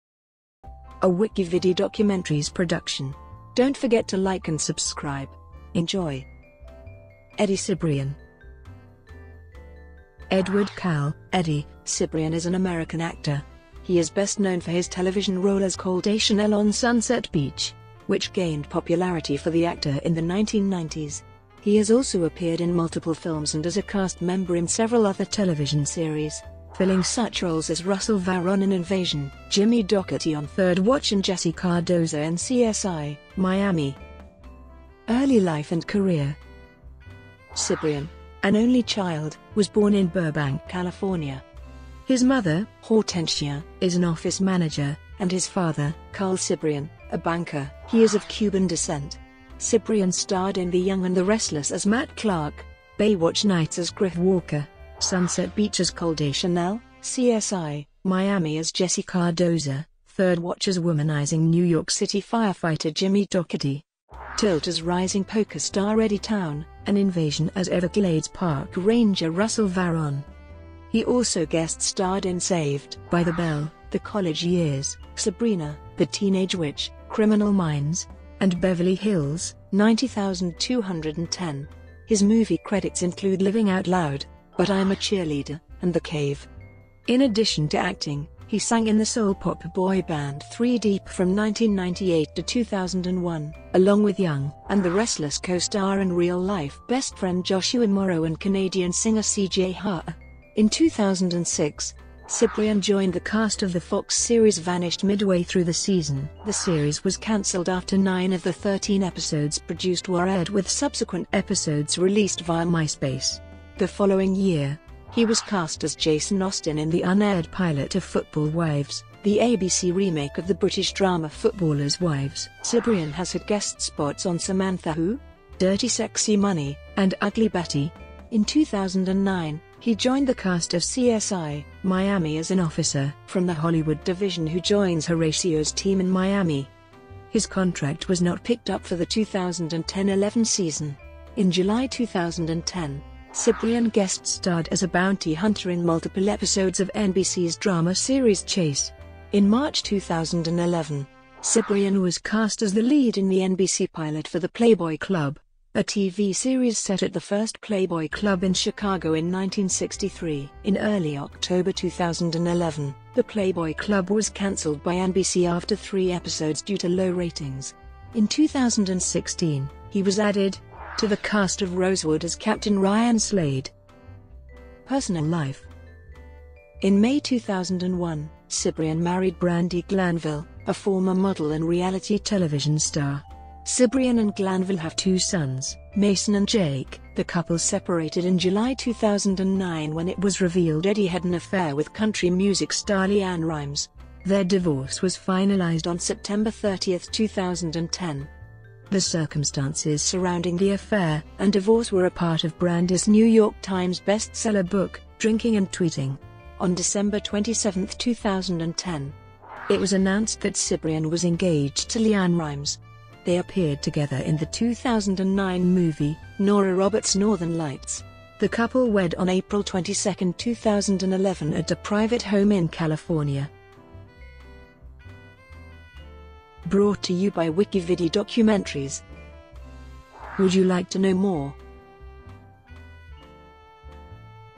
EDDIE CIBRIAN - WikiVidi Documentary
Edward Carl "Eddie" Cibrian is an American actor. He is best known for his television role as Cole Deschanel on Sunset Beach, which gained popularity for the actor in the 1990s. He has also appeared in multiple films and as a cast member in several other television series, filling such roles as Russell Varon in Invasion, Jimmy Doherty on Third Watch and Jesse Cardoza in CSI: Miami.

Shortcuts to chapters:
00:00:34 Early life and career

Copyright WikiVidi.